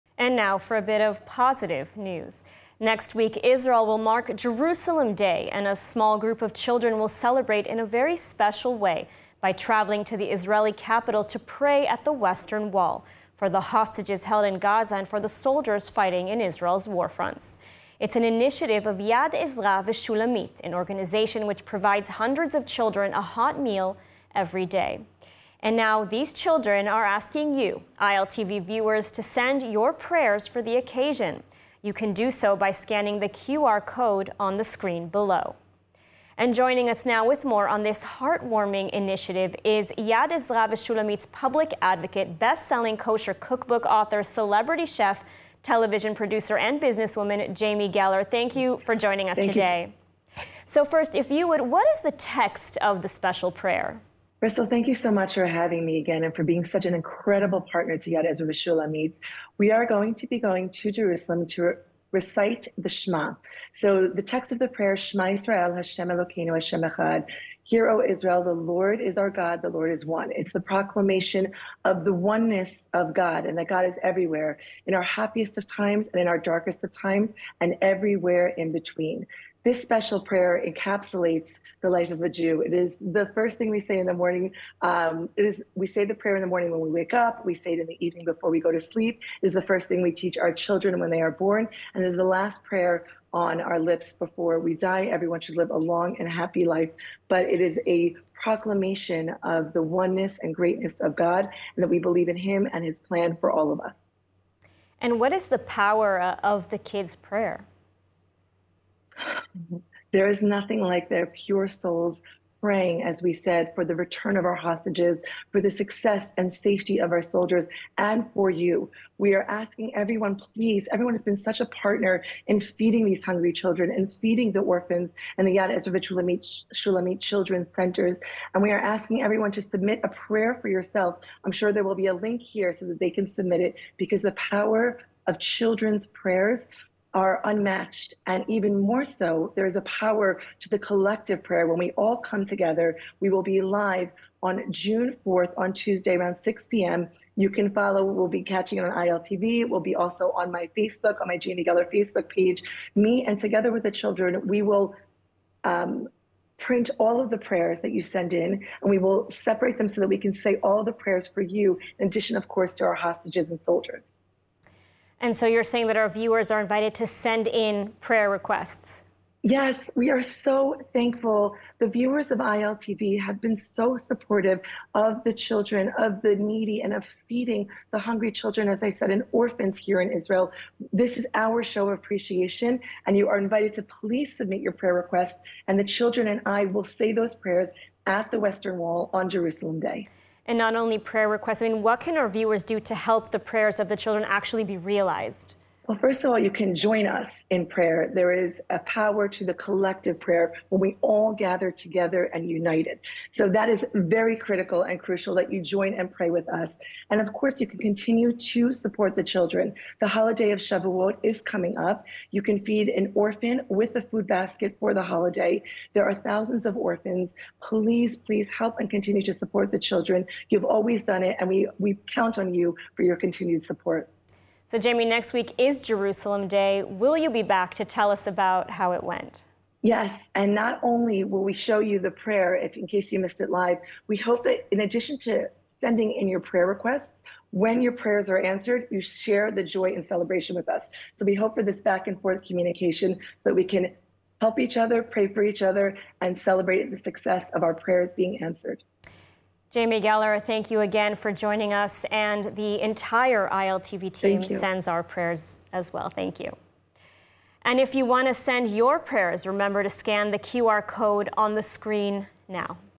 Jerusalem Day Prayer for Hostages and IDF
ILTV is joined by celebrity chef Jamie Geller to hear about Yad Ezra’s plan to bring 665 kids to Jerusalem to pray for the hostages and the IDF ahead of Jerusalem Day.